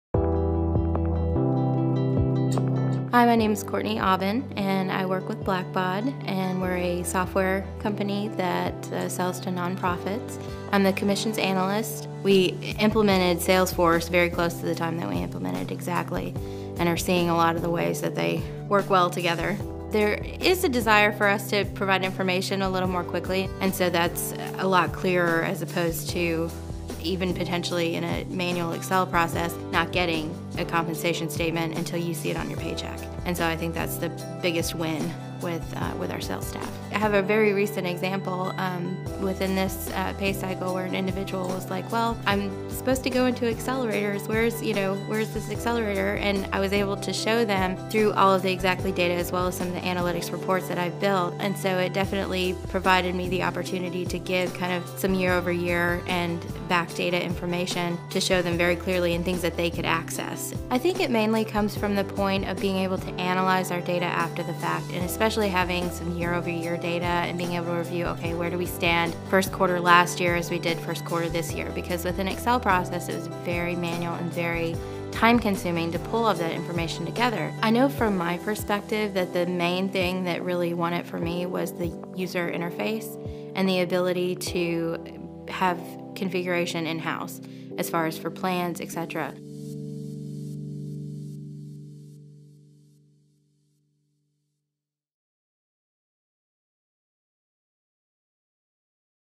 Blackbaud and Xactly | Customer Stories
Blackbaud's Commissions Analyst, Courtney Aubin discusses how Xactly drives efficiency, clarity, and more in their business processes.

Have more questions about Xactly and what we do?